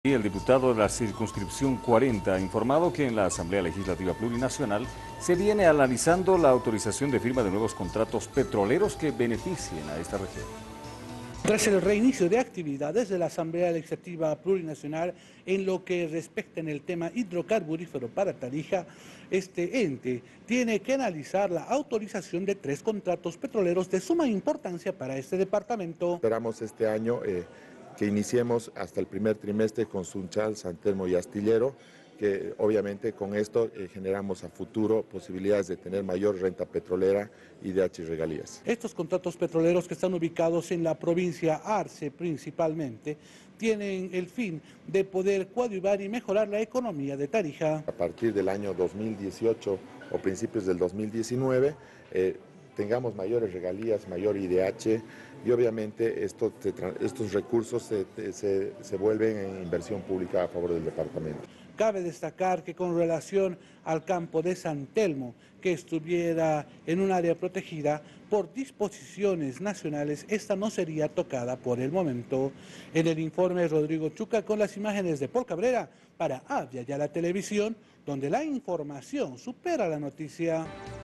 Tarija espera la aprobación de contratos petroleros de exploración y explotación -13 ene 2017
(Abya Yala Digital).- Este 2017 la Asamblea Legislativa Plurinacional debe aprobar tres contratos petroleros de exploración y explotación que beneficirán a Tarija.(video)